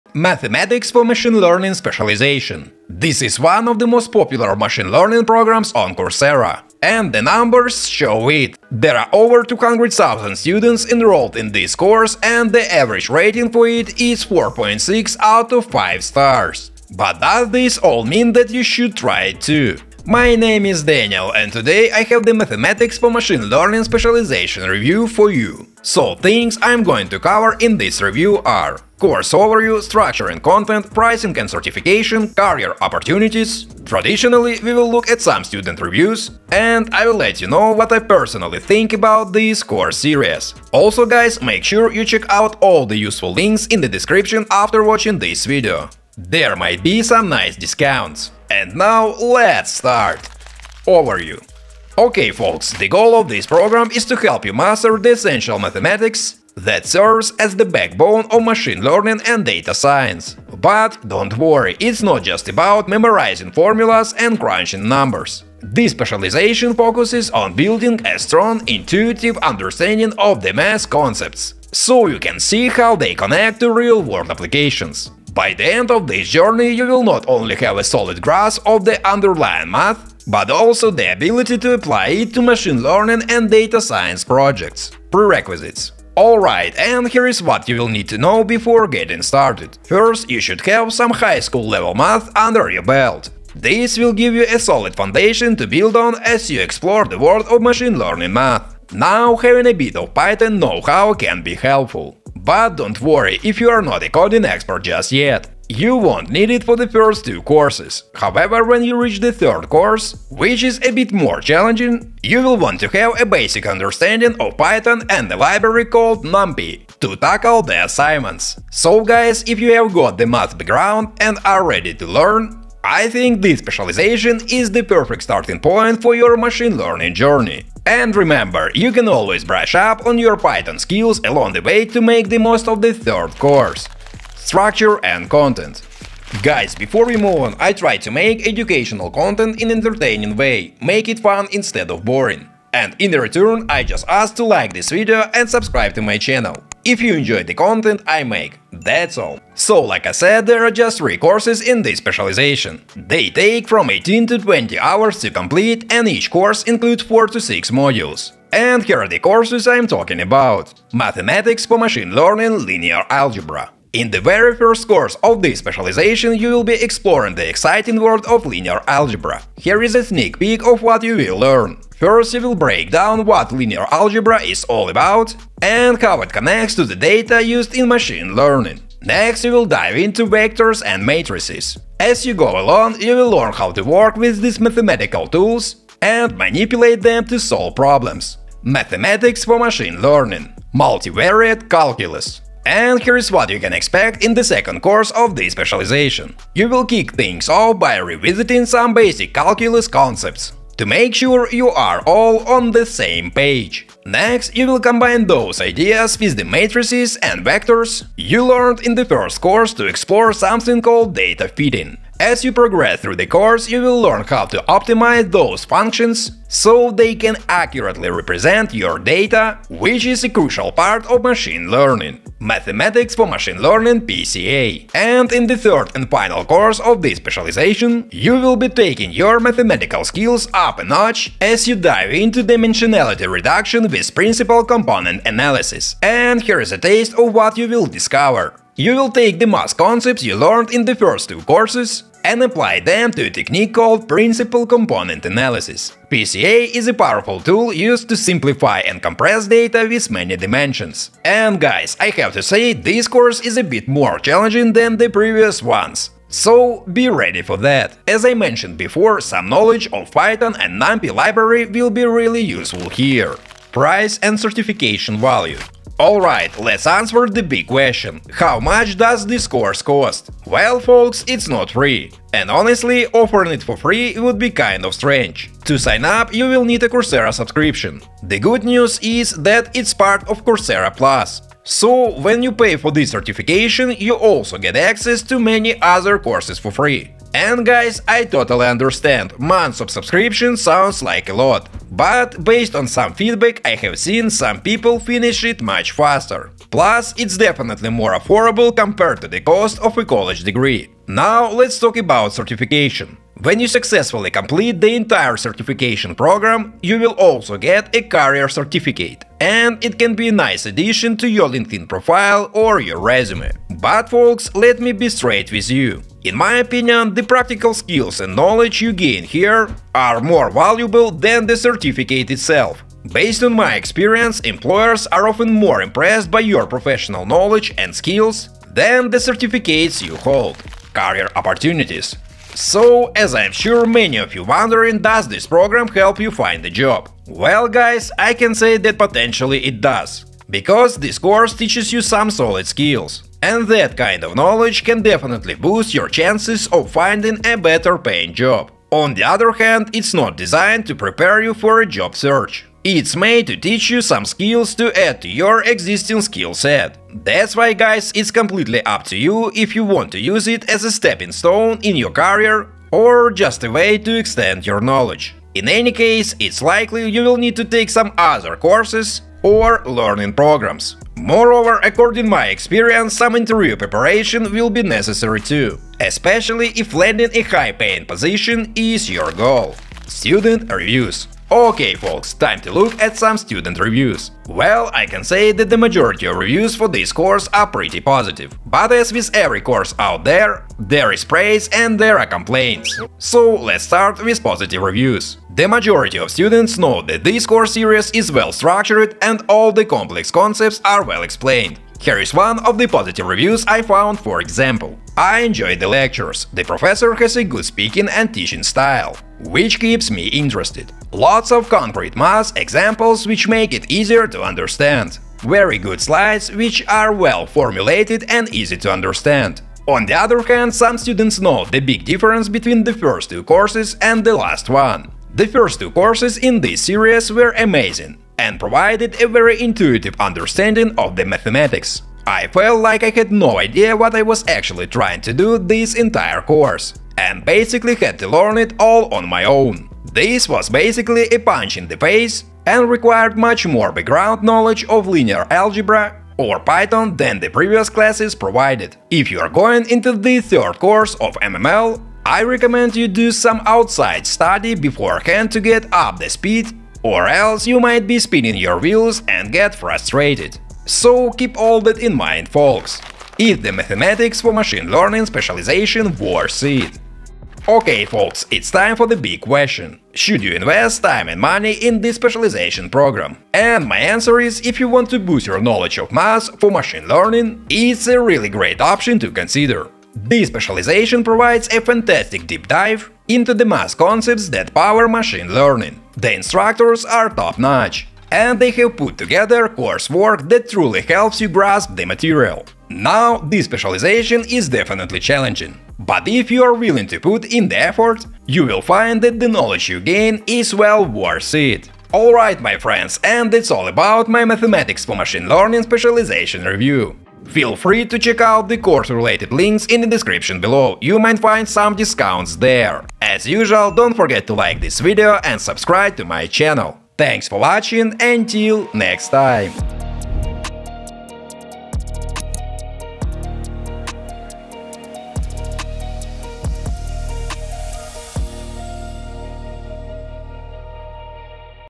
Mathematics for Machine Learning Specialization Review - 2025 | Is it Worth it? (Coursera Review)
Today, I'll make the Mathematics for Machine Learning Specialization Review. This course series was created by Imperial College London and placed on Coursera. In this video, I'll discuss the course overview, structure and content, pricing, certification value, career opportunities, and student reviews. I'll also share my personal thoughts about this course series.

⏱️ CHAPTERS:
0:00 Mathematics for Machine Learning Specialization Review
0:52 Overview
1:25 Prerequisites
2:15 Structure and Content
4:24 Price and Certification
5:31 Career Opportunities
6:21 Student Reviews
7:44 Is Mathematics for Machine Learning Specialization Worth it?